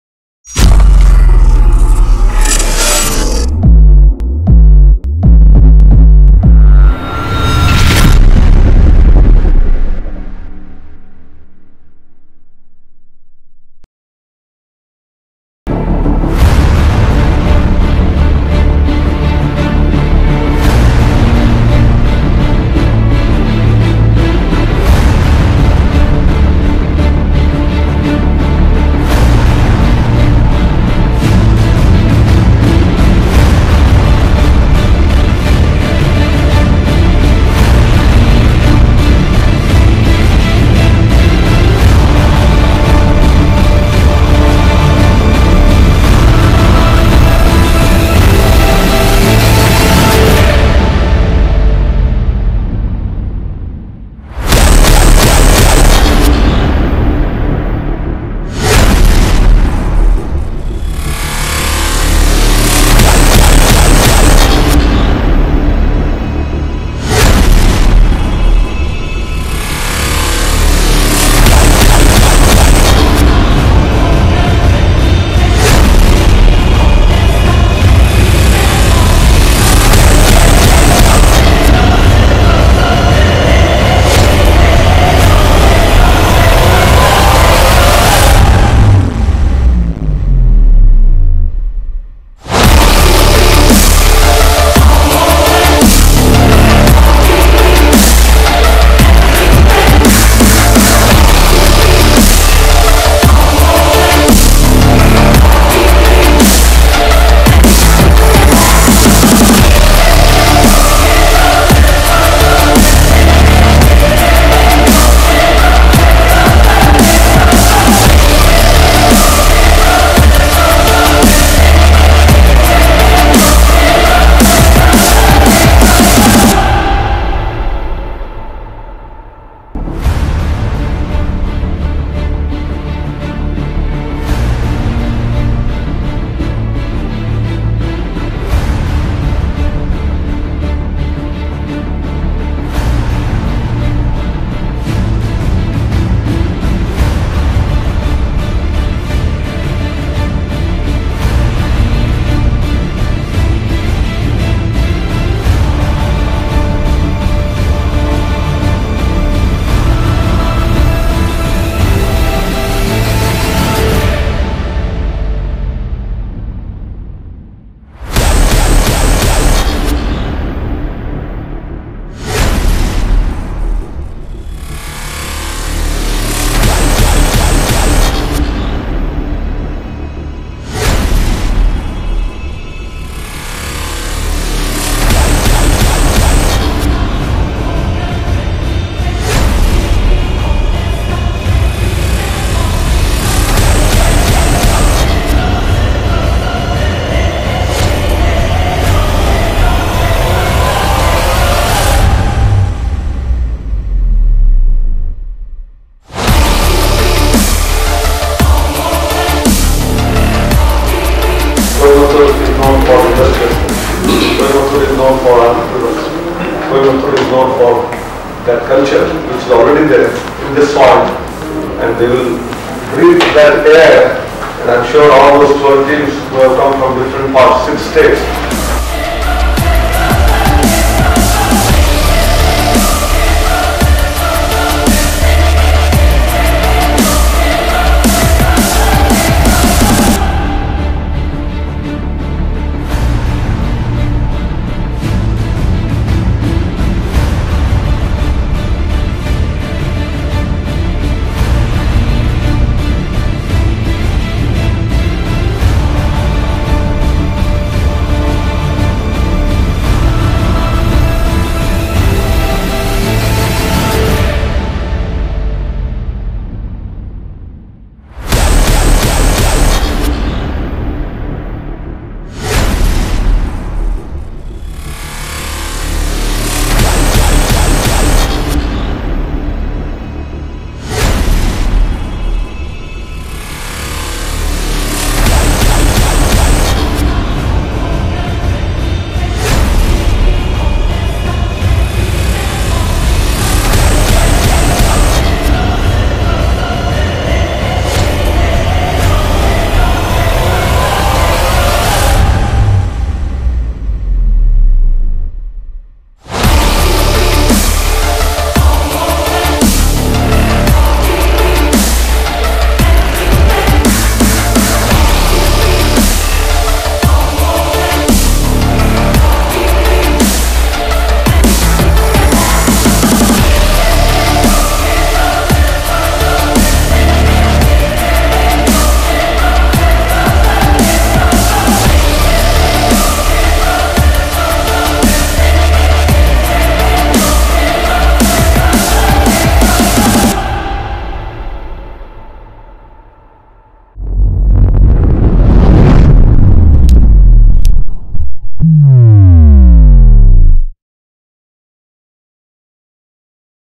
Smart India Hackathon 2019 - Hardware Edition @ FORGE Accelerator | Highlights
Highlights video of the Smart India Hackathon 2019 - Hardware Edition @ FORGE Accelerator, Coimbatore